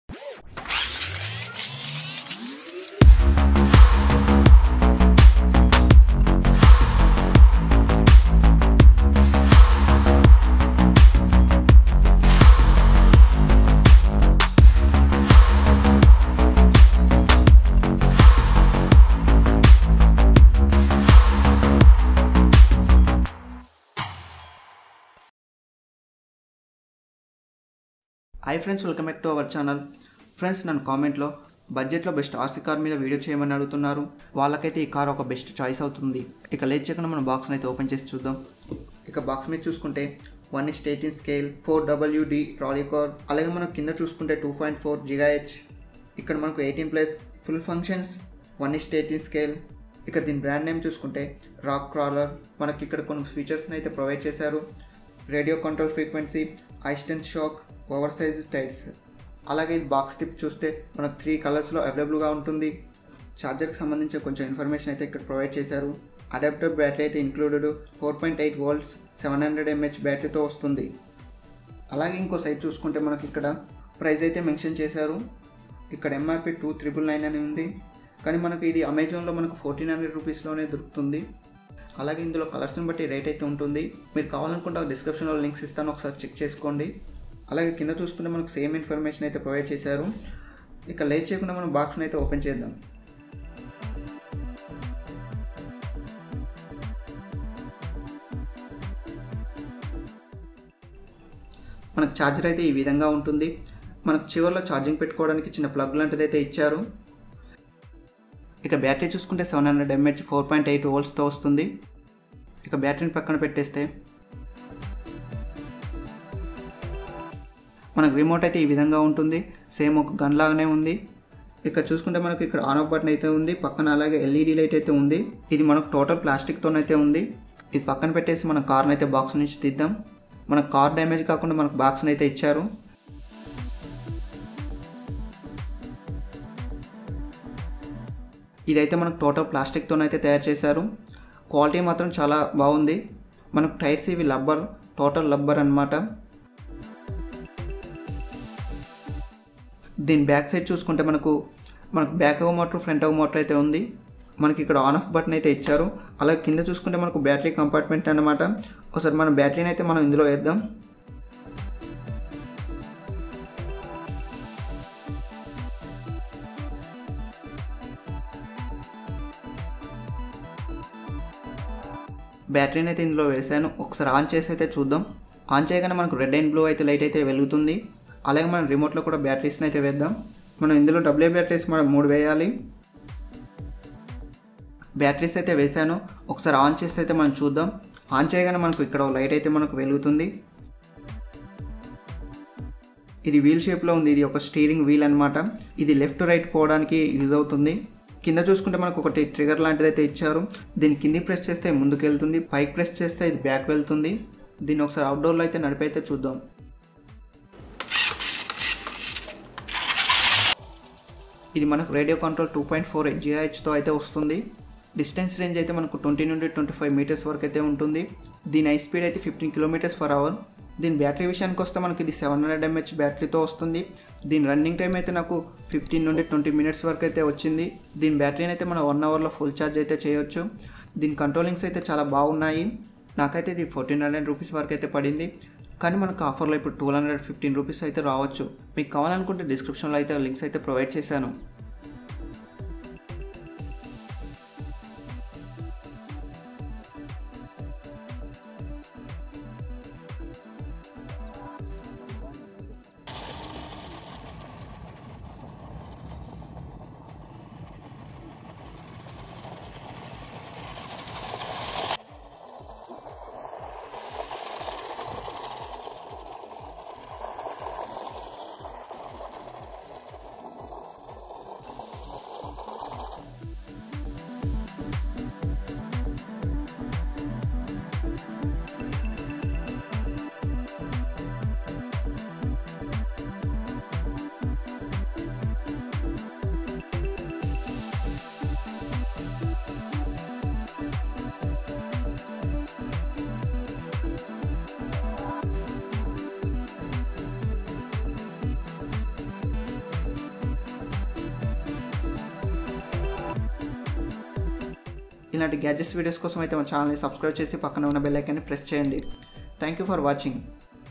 RC Rock Crawler Rally Car Unboxing & Testing In Telugu
RC Rock Crawler Car .(Amazon) Likns

RC Rock Crawler Rally Car (Green)

RC Rock Crawler Rally Car (Red)

RC Rock Crawler Rally Car (Blue)